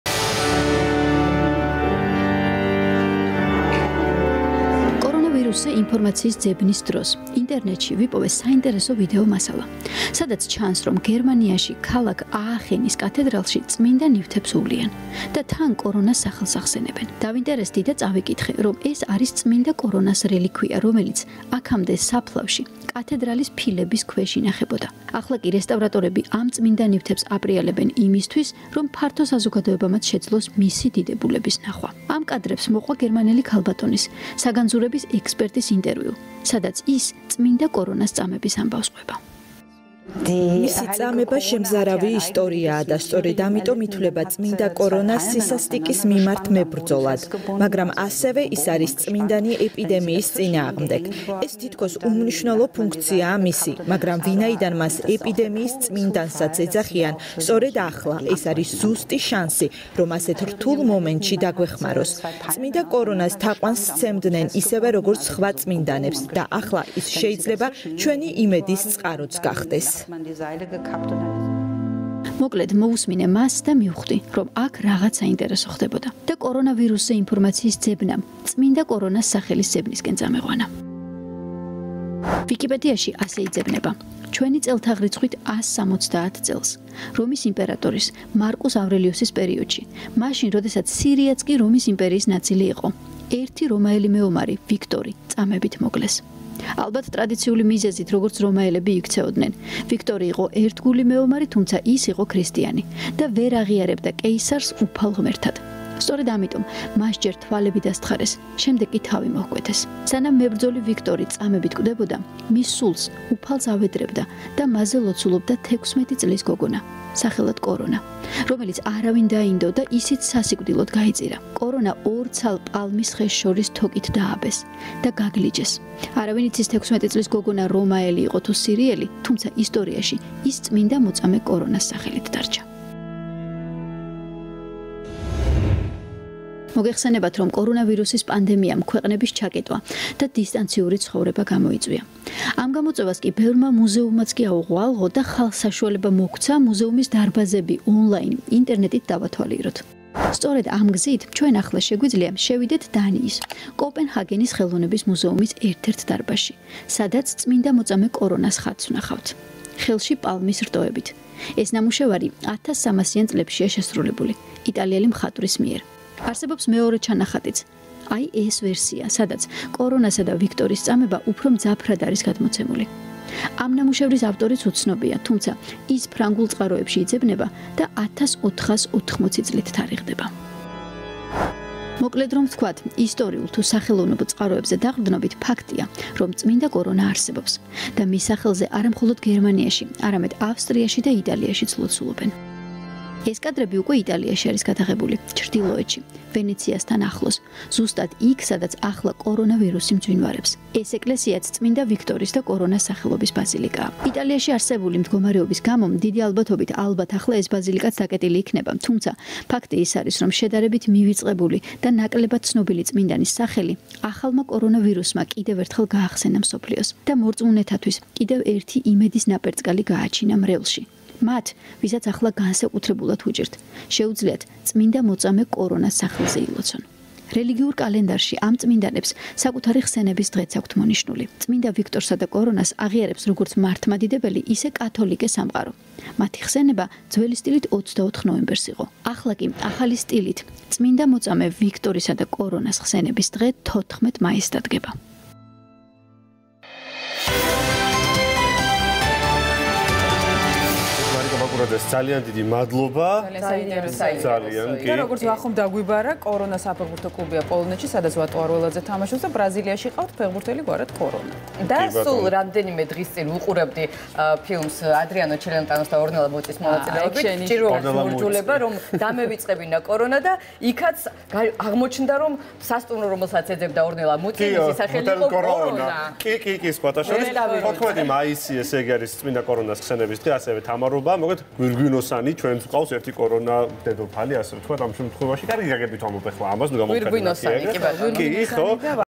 წმინდა მოწამე კორონა - მართლმადიდებელი და კათოლიკური სამყაროს წმინდანი
მინდანი, რომელსაც მართლმადიდებელი და კათოლიკური სამყარო ერთად აღიარებს.